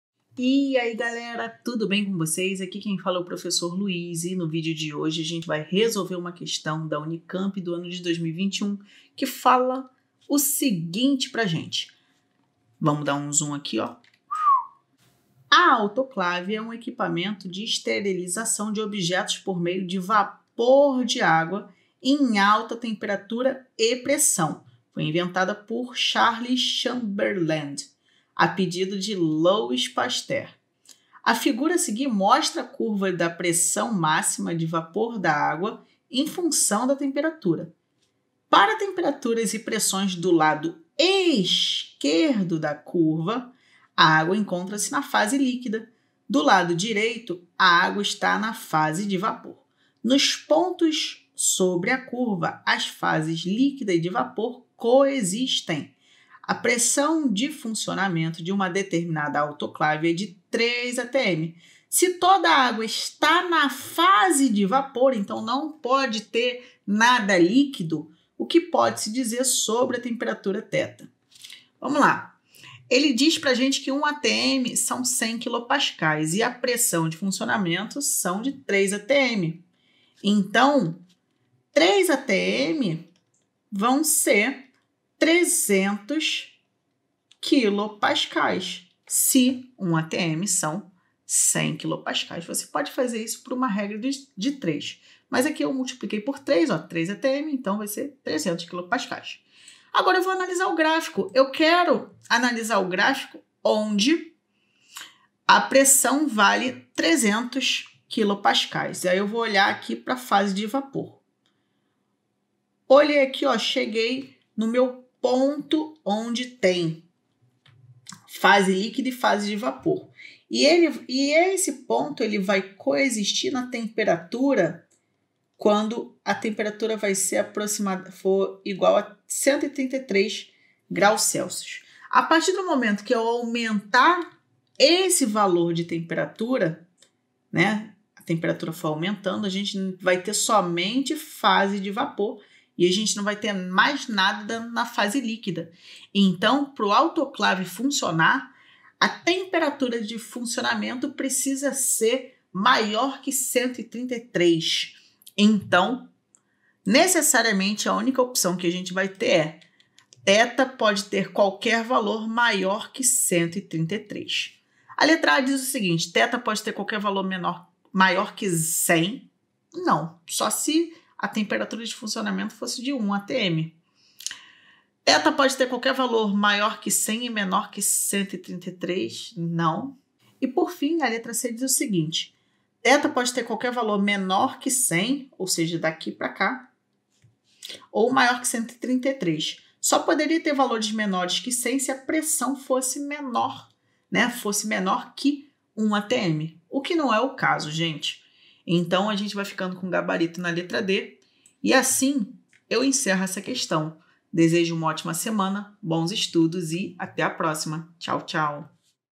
UNICAMP 2021 - A autoclave, um equipamento de esterilização de objetos por meio de vapor de água em
A autoclave, um equipamento de esterilização de objetos por meio de vapor de água em alta temperatura e pressão, foi inventada por Charles Chamberland, a pedido de Louis Pasteur...

A figura a seguir mostra a curva da pressão máxima de vapor da água em função da temperatura.
Para temperaturas e pressões do lado esquerdo da curva, a água encontra-se na fase líquida; do lado direito, a água está na fase de vapor. Nos pontos sobre a curva, as fases líquida e de vapor coexistem. A pressão de funcionamento de uma determinada autoclave é p = 3,0 atm.

Se toda a água está na fase de vapor, o que se pode dizer sobre a sua temperatura θ ?

Unicamp 2021 - Questão 62

Referências
Imagem do Logo do Insta